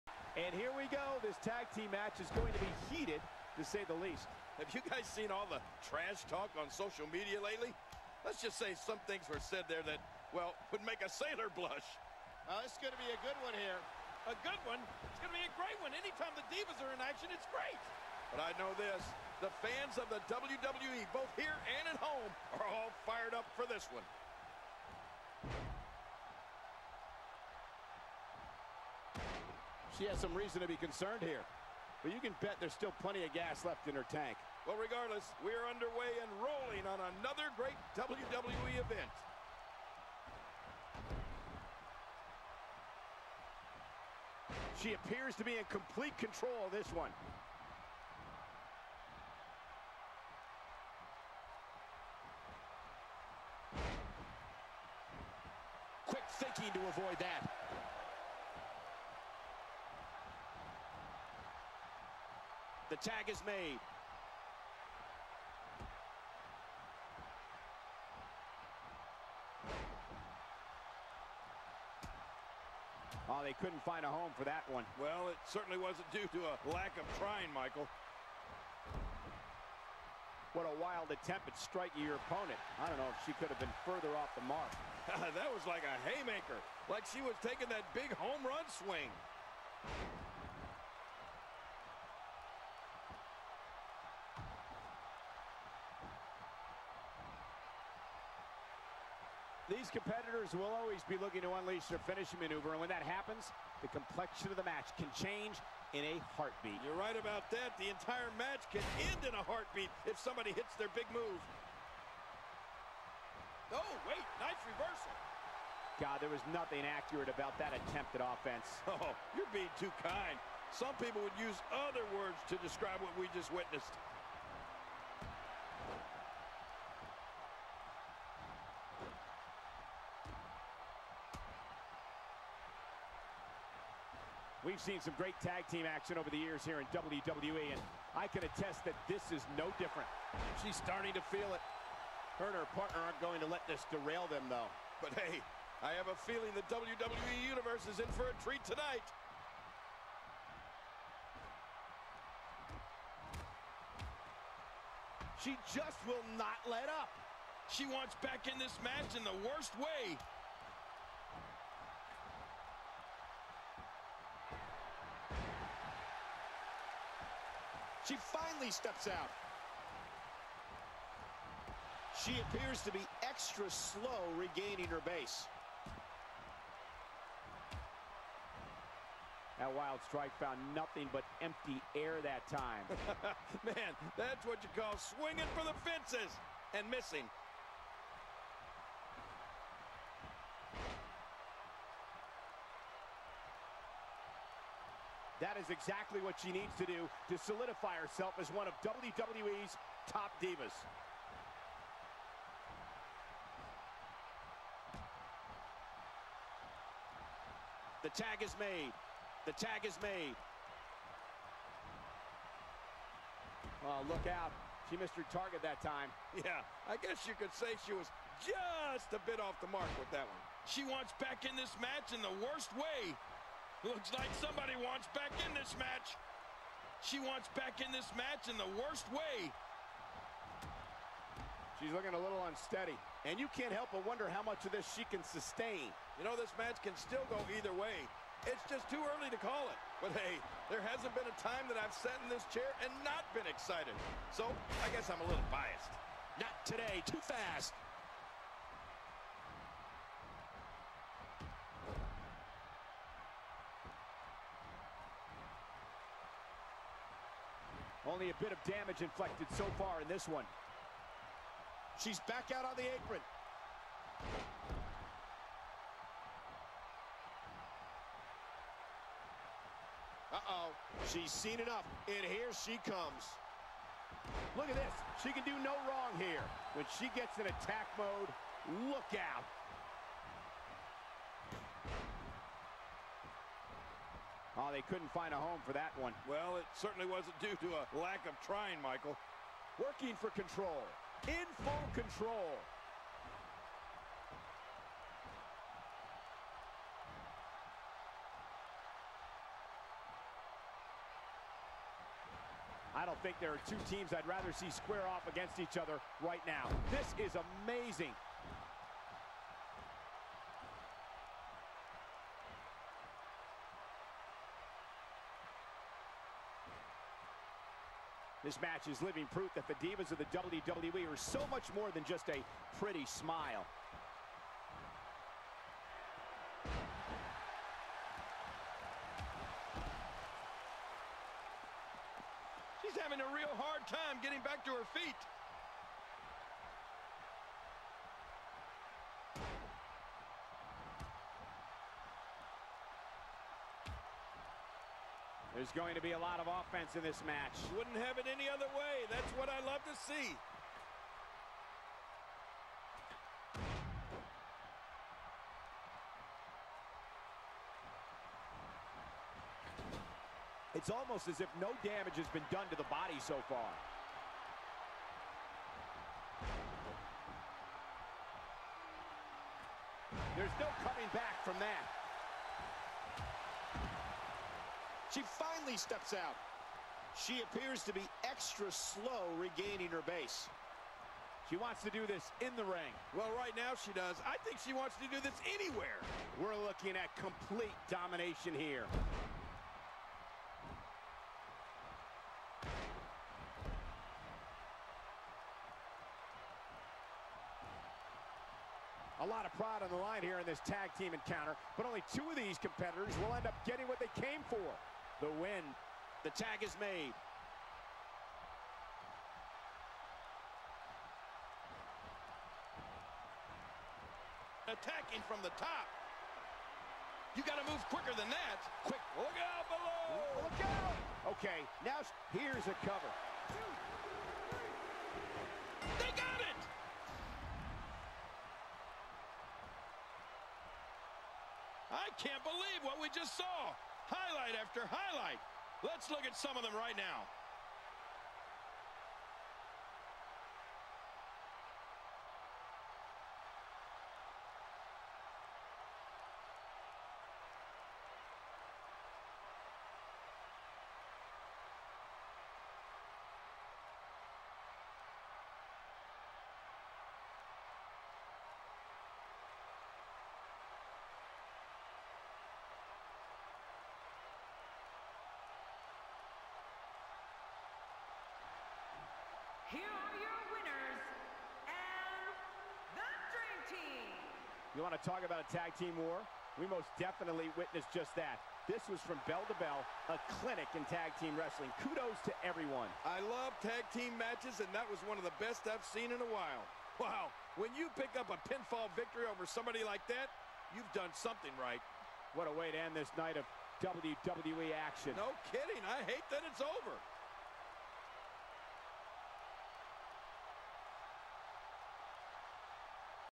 2K17 : Bev & Rivka vs. Kayla & Marisol (3rd fall)
The Jewish girls strike back, with Bev pinning Kayla, while Rivka successfully protects the pin by blocking Marisol's dropkick. Now it's all even, and anyone's match for the taking.